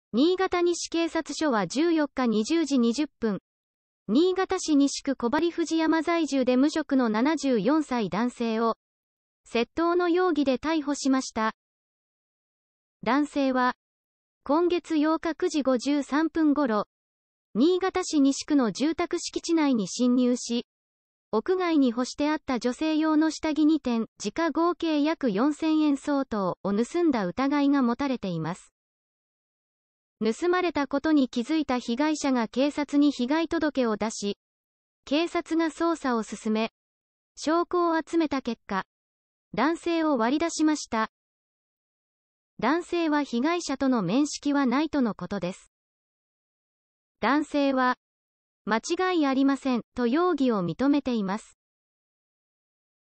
【逮捕】住宅敷地内に侵入して女性用下着を盗んだ新潟市西区在住の男性を逮捕
新潟西警察署は１４日２０時２０分、新潟市西区小針藤山在住で無職の７４歳男性を、窃盗の容疑で逮捕しました。

男性は、今月８日９時５３分頃、新潟市西区の住宅敷地内に侵入し、屋外に干してあった女性用の下着２点（時価合計約４，０００円相当）を盗んだ疑い。

盗まれたことに気付いた被害者が警察に被害届けを出し、警察が捜査を進め、証拠を集めた結果、男性を割り出しました。男性は被害者との面識はないとのことです。

男性は、「間違いありません」と容疑を認めています。

【にいがた経済新聞】にいがたをもっと身近に。